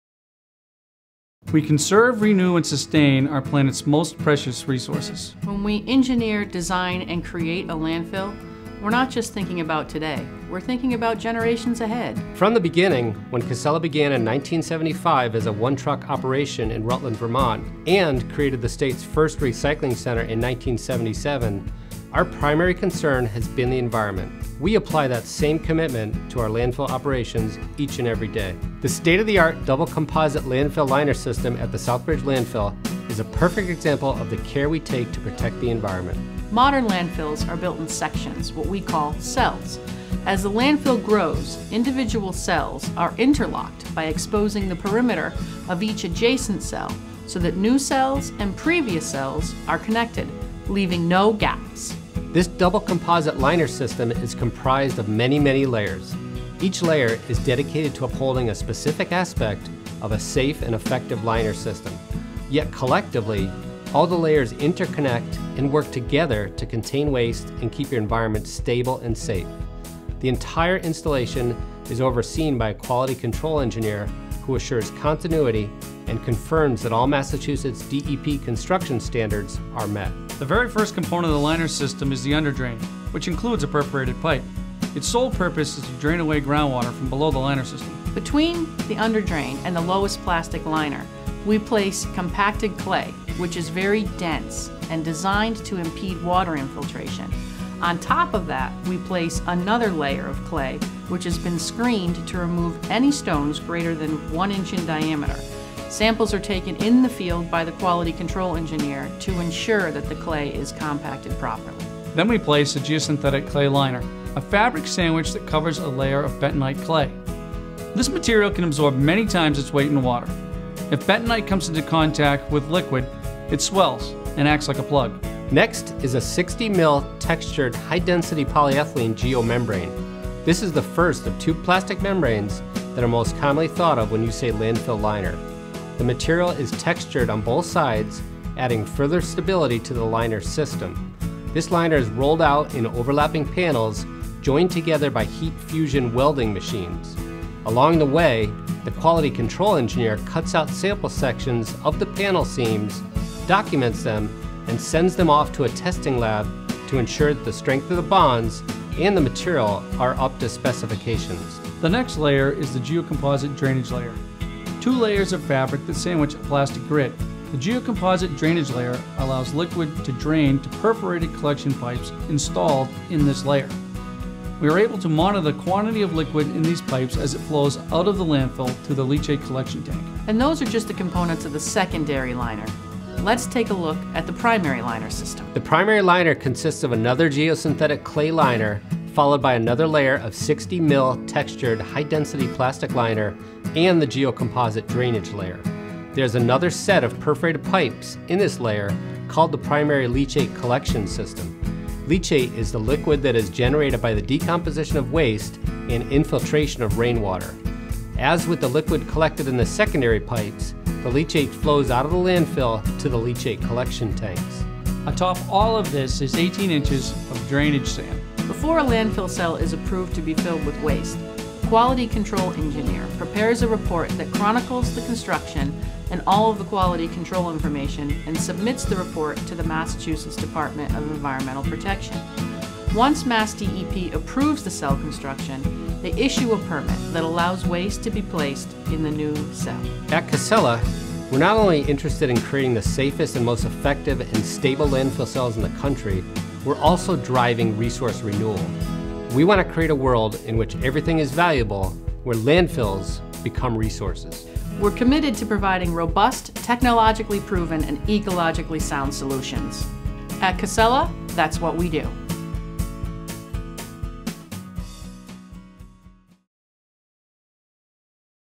Modern Landfills: Engineered and Managed to Protect the Environment
When we engineer, design and create a landfill, we aren't just thinking about today, but we are thinking about generations ahead. Take a look into how we develop our landfills with protecting the environment at the forefront.